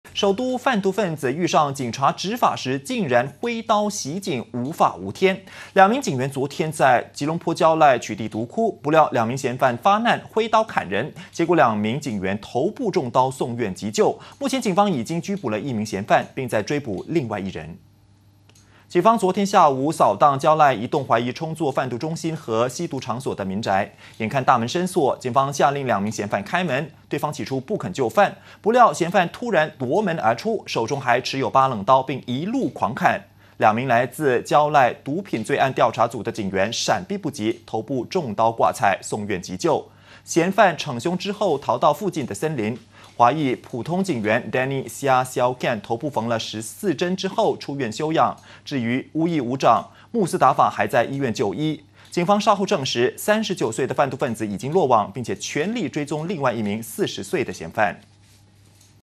蕉赖毒贩拒捕挥刀袭警 2警员遇袭头部缝14针 [20160430]
首都贩毒分子遇上警察执法时，竟然挥刀袭警无法无天。

两名警员昨天在吉隆坡蕉赖，取缔毒窟，不料2名嫌犯发难，挥刀砍人，结果两名警员，头部中刀，送院急救。目前警方已经拘捕1名嫌犯，并在追捕另外1人。